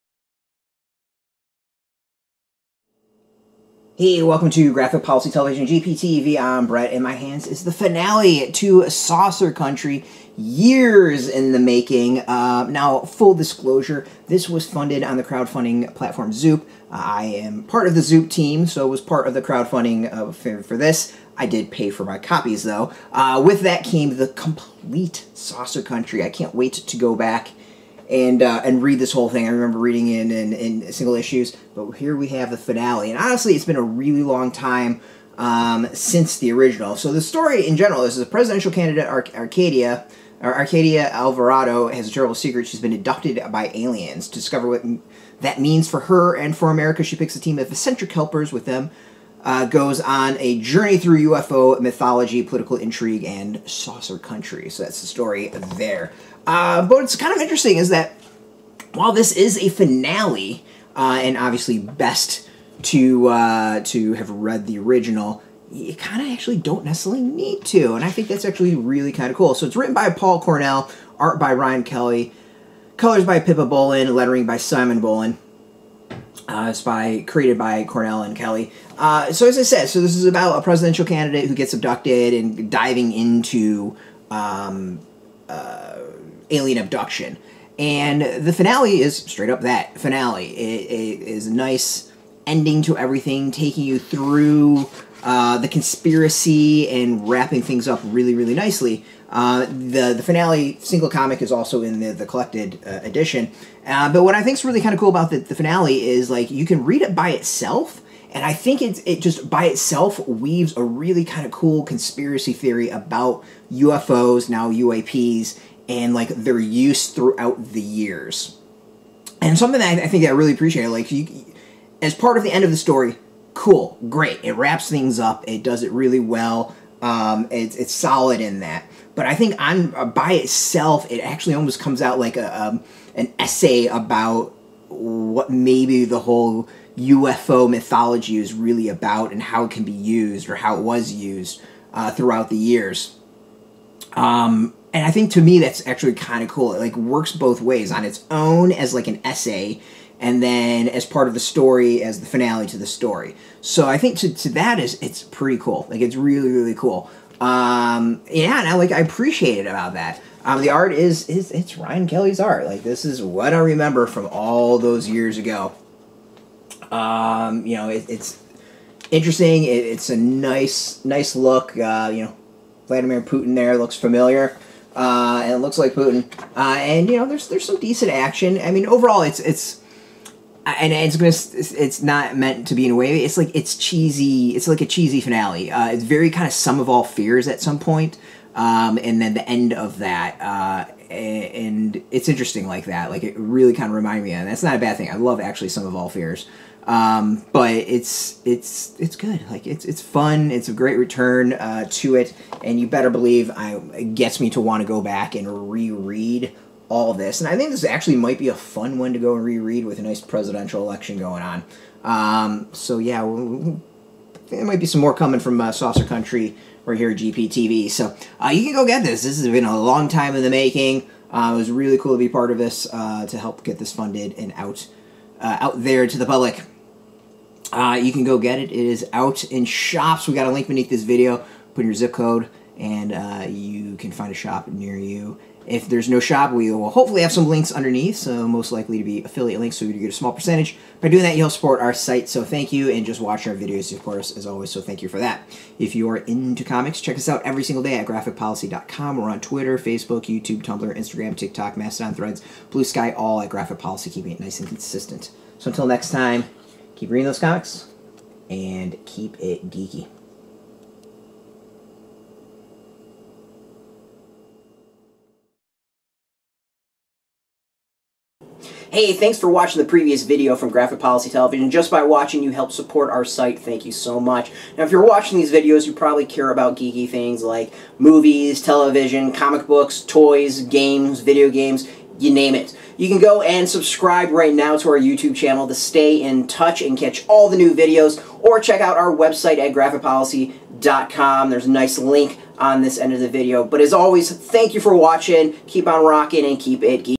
Saucer Country: The Finale wraps things up for long time fans but is fun on its own too
First there was Saucer Country, a dark thriller that blended UFO lore and alien abduction with political intrigue, all set in the hauntingly beautiful Southwest. The story continued in Saucer State... and now, the series reaches its long-awaited conclusion in this special one-shot, as co-creators Cornell and Kelly present the final story of alien abductee Arcadia Alvarado's campaign to be US President... and her search for the truth of what happened to her!

Story: Paul Cornell
Art: Ryan Kelly
Color: Pippa Bowland
Letterer: Simon Bowland